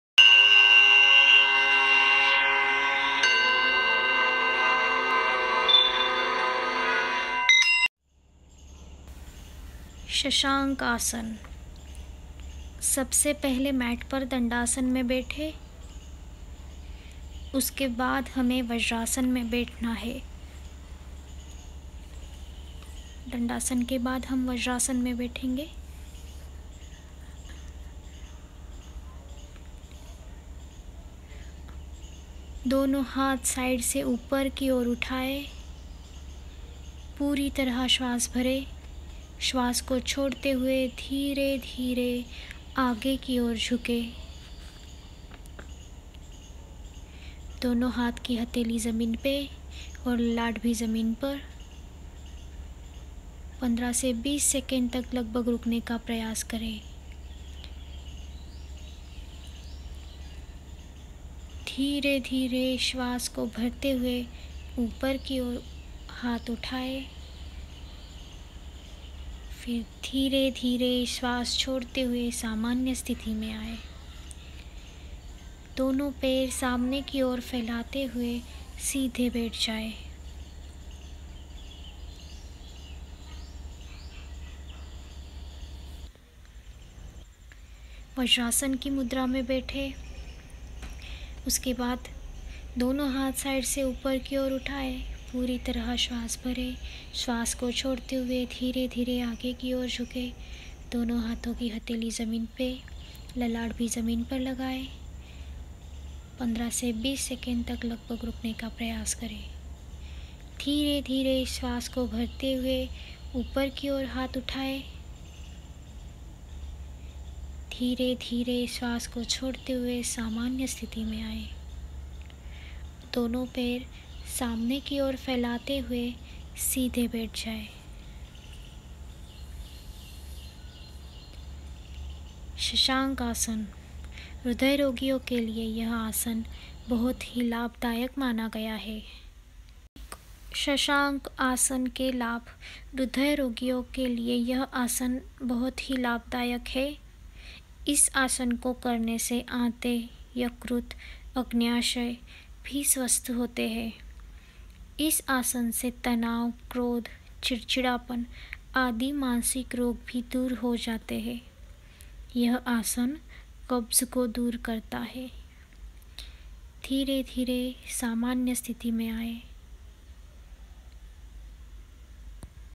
How to do shashanksasana | शशांकासना | Dr Tejashri’s Yoga
Hello everyone,

 I m Dr Tejashri Pathak Joshi ,

शशांकासना के नियमित अभ्यास से हृदय रोगों के अलावा अनेक रोगों से बचा जा सकता हे | शशांक का अर्थख़रगोश होता हे | इस आसन में बेठते समय व्यक्ति की मुद्रा ख़रगोश के समान हो जाती हे , इसलिए इसे शशांकासन कहते हे ।